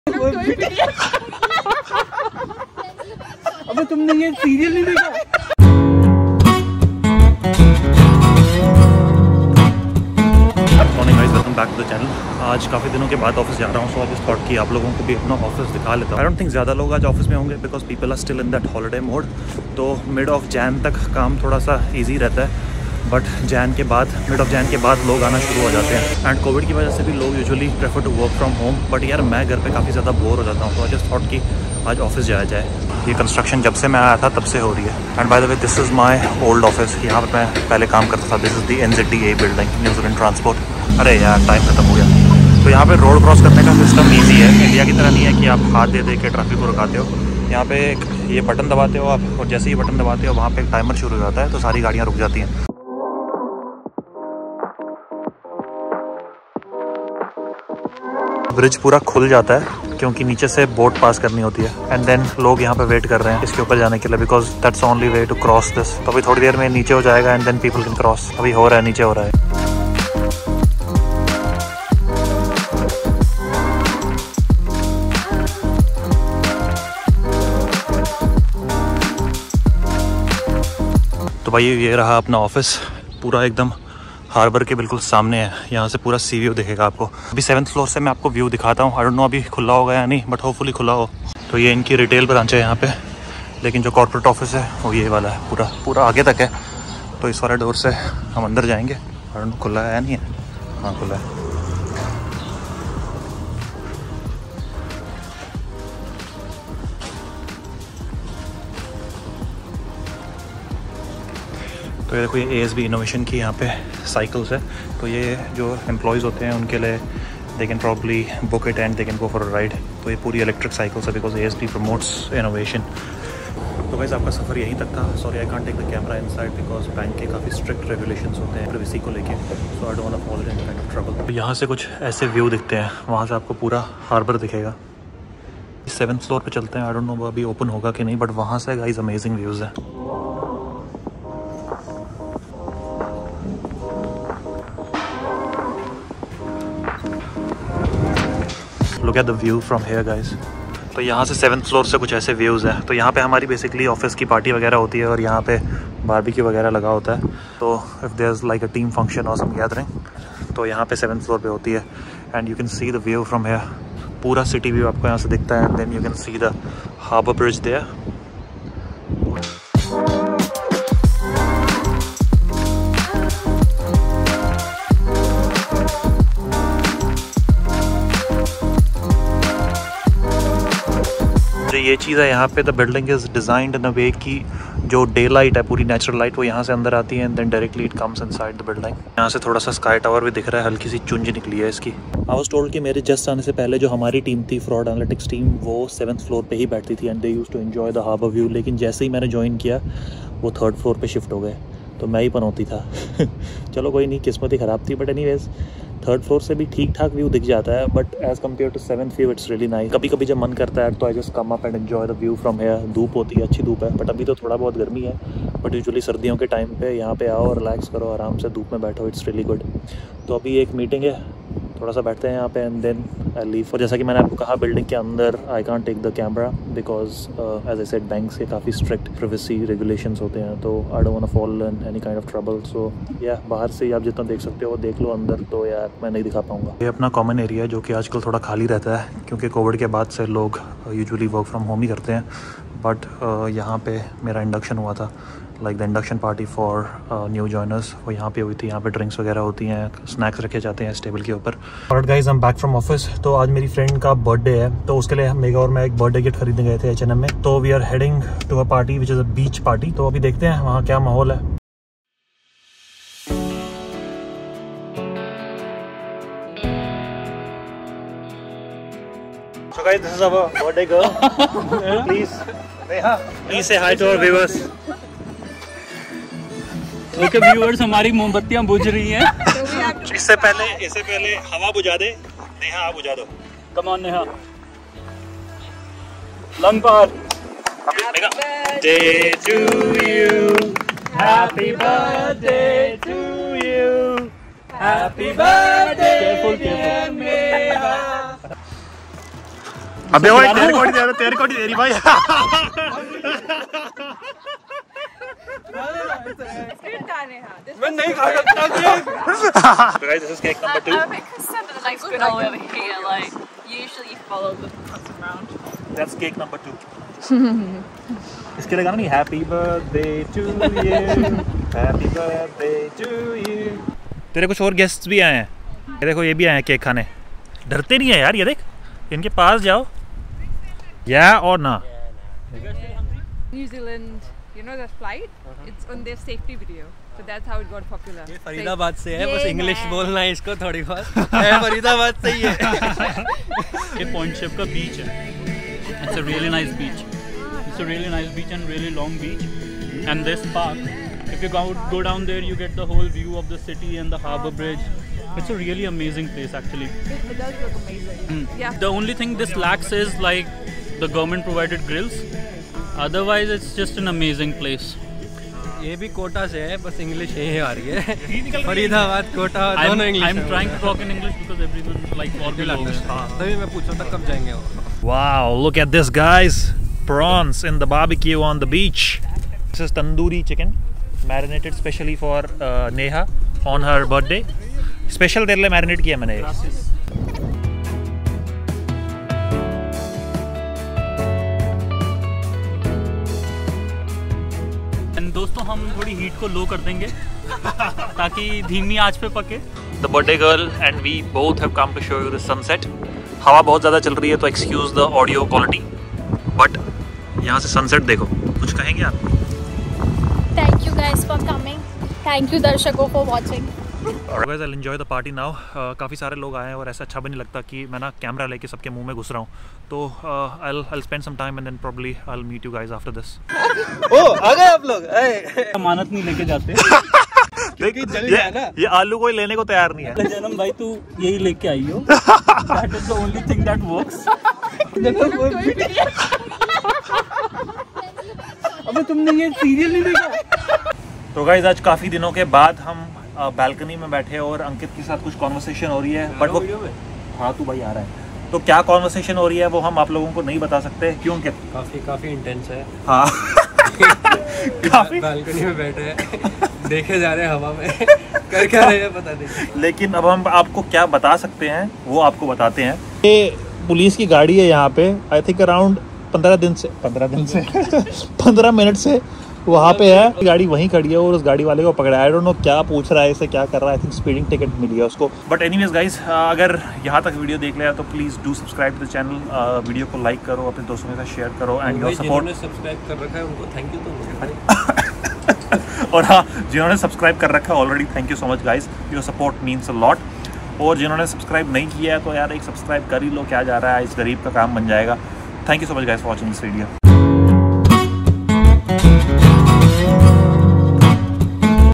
Visit to ASB Bank Auckland City Central | Beach Party | West Auckland
Visited ASB North Wharf Office followed by a beach party in West Auckland New Zealand.